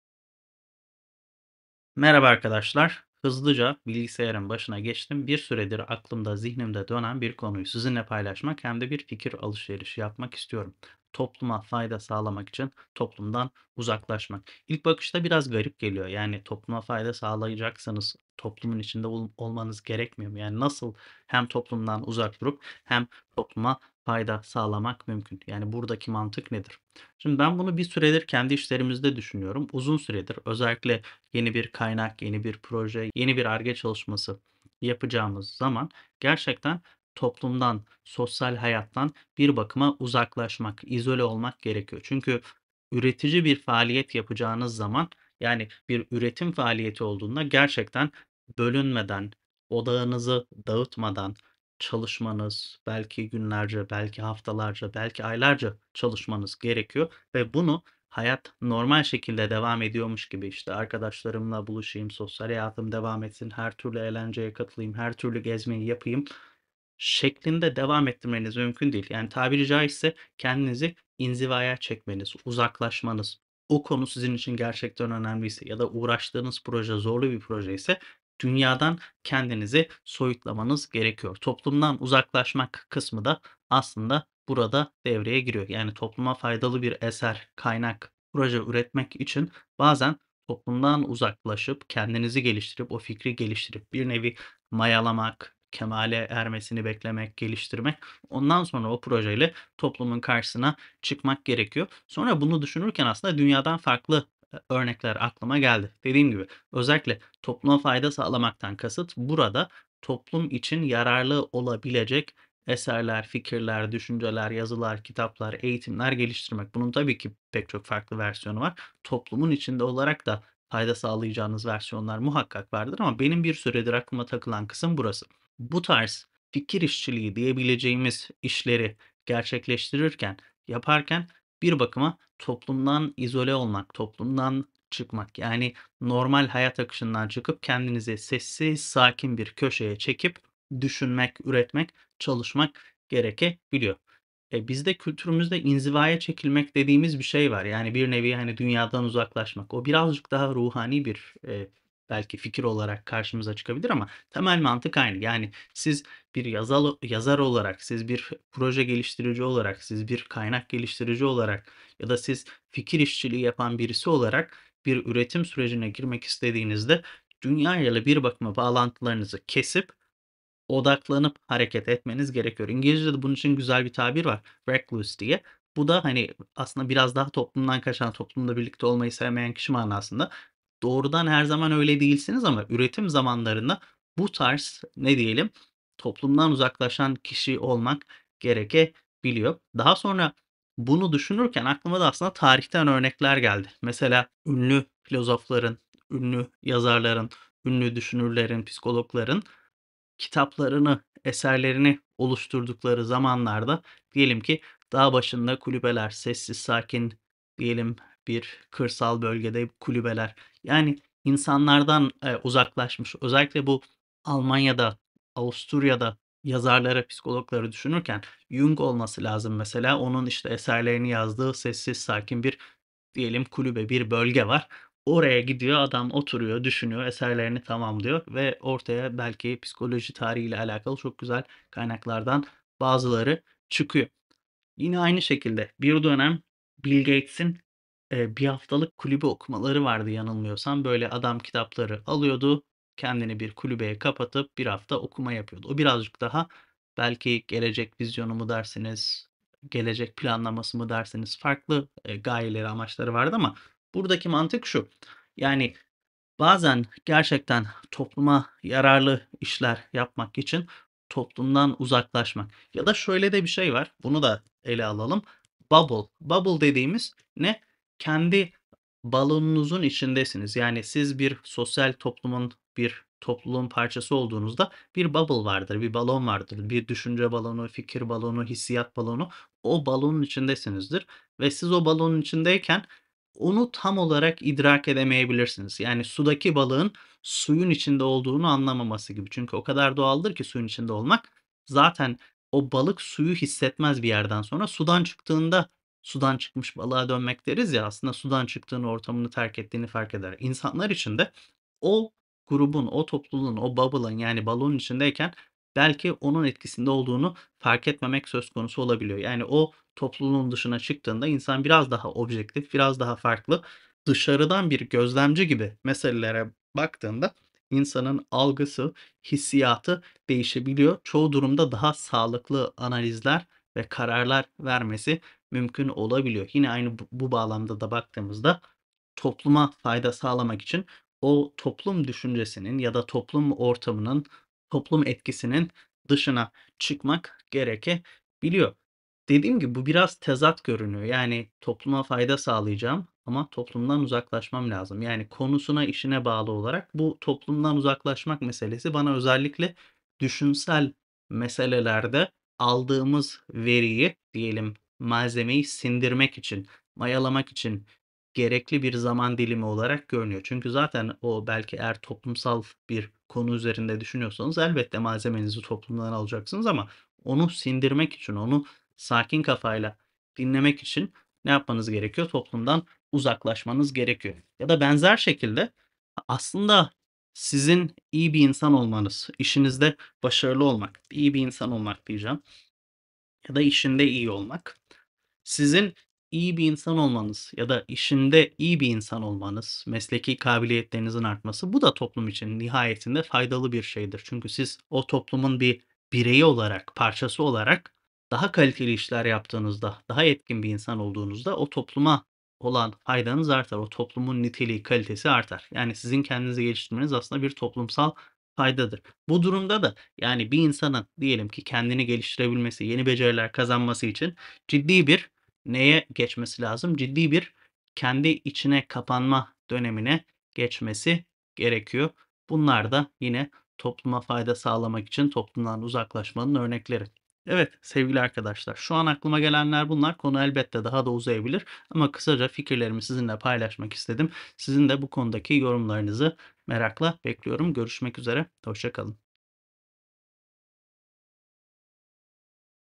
Topluma Fayda Sağlamak için Toplumdan Uzaklaşmak
Topluma fayda sağlayacak işler yapmak için bazen toplumdan uzaklaşmak gerekiyor. Bugün bir süredir aklımdaki bu konuyu sizinle de paylaşmak istedim. Yorumlarınızı bekliyorum.